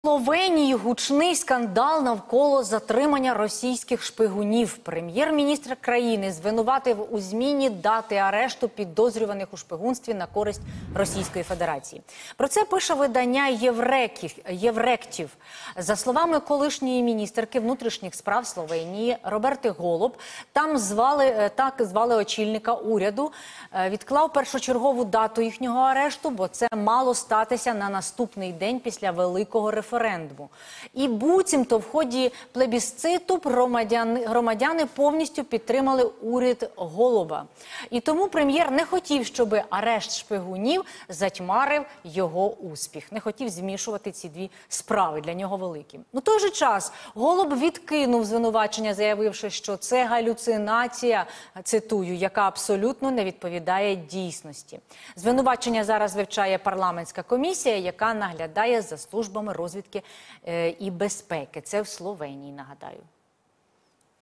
Прем'єр Словенії опинився у центрі скандалу із затриманням шпигунів РФ
Затримання двох імовірних російських шпигунів у Словенії в листопаді 2022 року стало основою для звинувачень у бік прем'єр-міністра Роберта Голоба у втручанні в роботу правоохоронних органів.